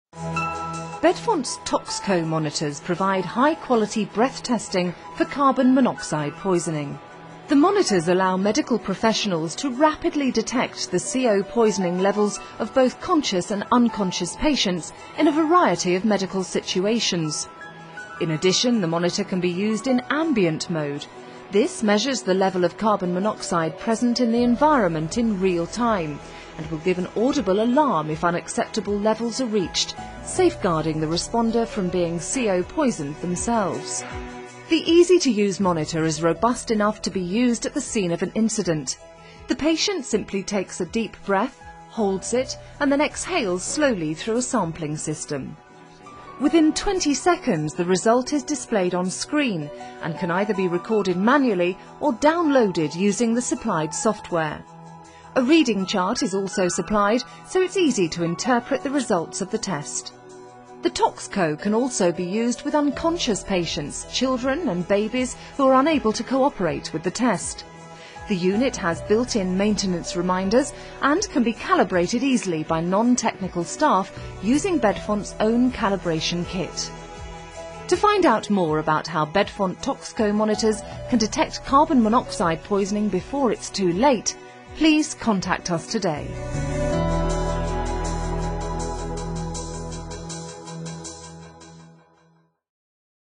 Bedfont ToxCO CO poisoning breath test
The ToxCO is a carbon monoxide detector that can be used to measure ambient carbon monoxide, and as a carbon monoxide breath test. Carbon monoxide poisoning can easily be fatal and so a portable carbon monoxide breath test, like the ToxCO, is a breakthrough lifesaving device. The portable carbon monoxide detector can diagnose carbon monoxide poisoning by analysing a single breath, and a facemask sampling system allows unconscious patients carbon monoxide levels to be measured. The carbon monoxide monitor is fully portable and easy to use, with carbon monoxide levels displayed clearly on the colour touch screen.